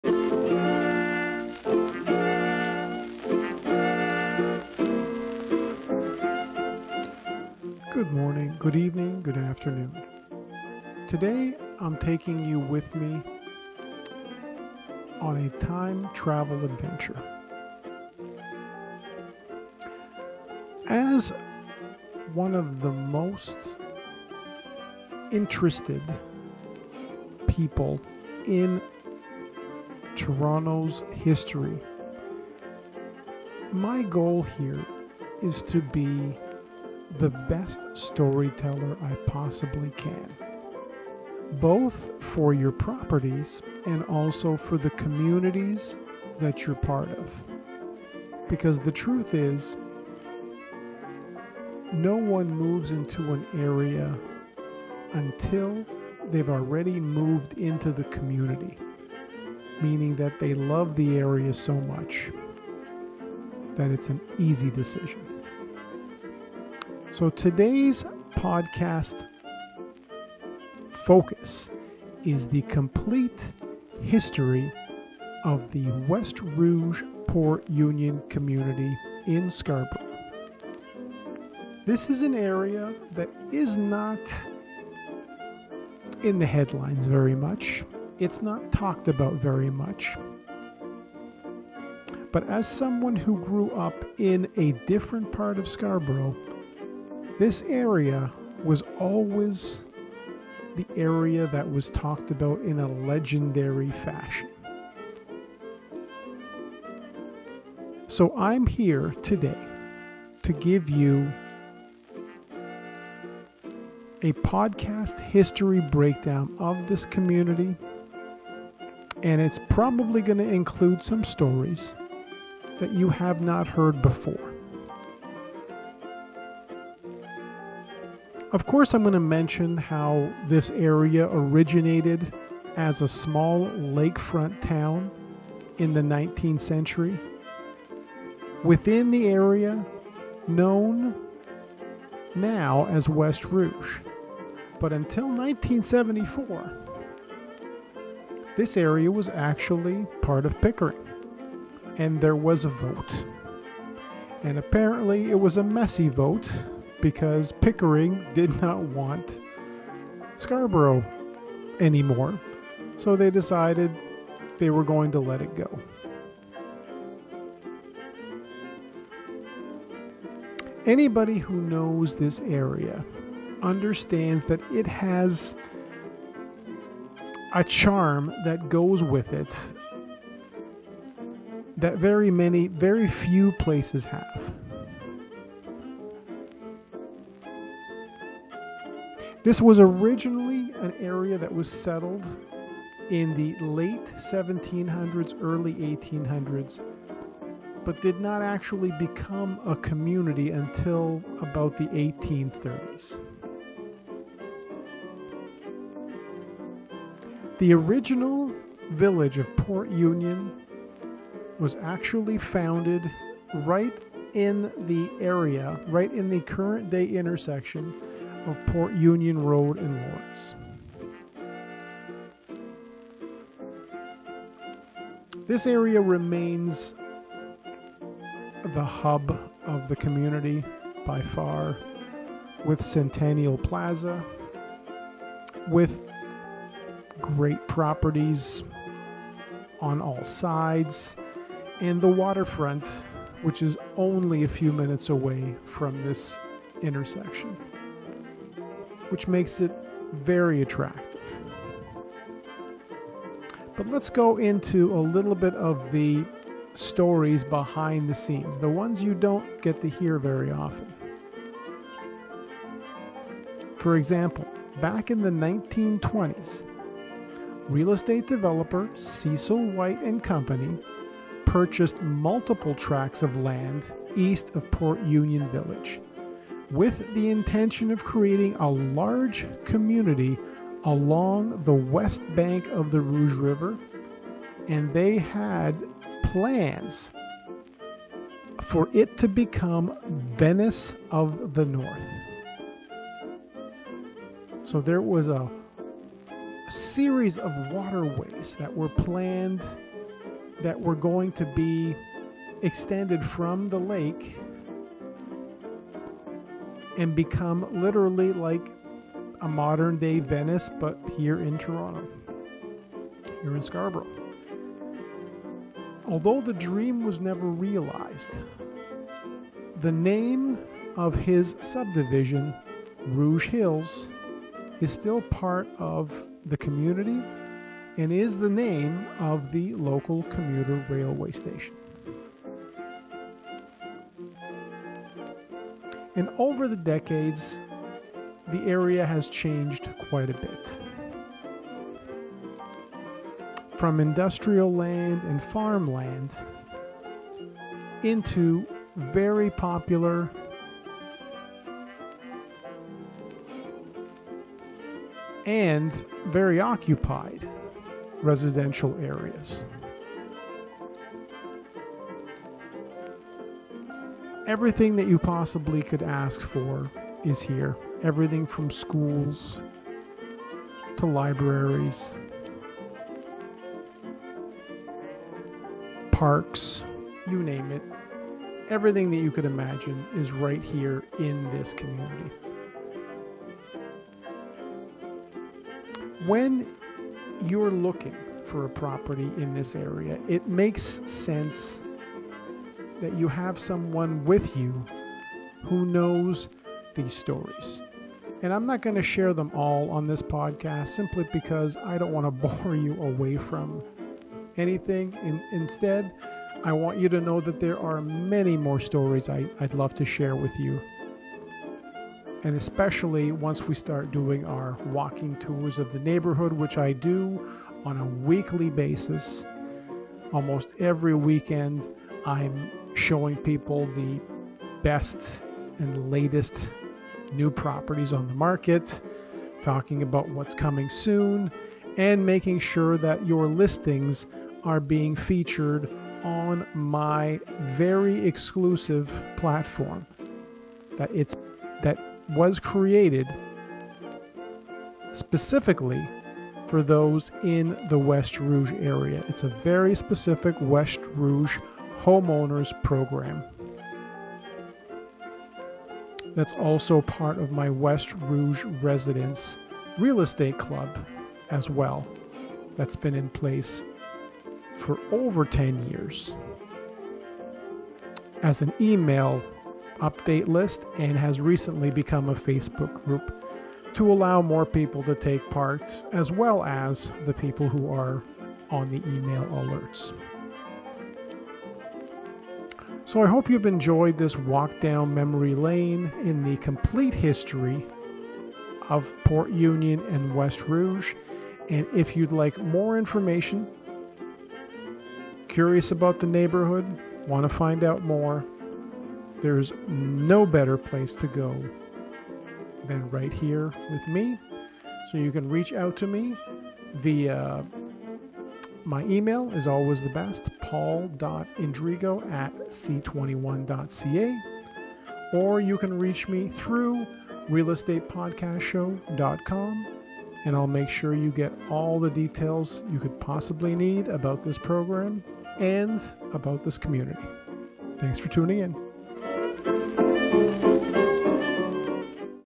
The Complete Podcast History of West Rouge and Port Union Community
Join me on my ongoing history series dedicated to the Toronto GTA areas that I know and love. Many of which I have called home over the last 50 years. This is one of those areas. Enjoy the stories and be sure to reach out if you have any questions at all or need helping moving into or out of this area.

Send in a voice message the VIP Podcast Club at RealEstatePodcastShow.com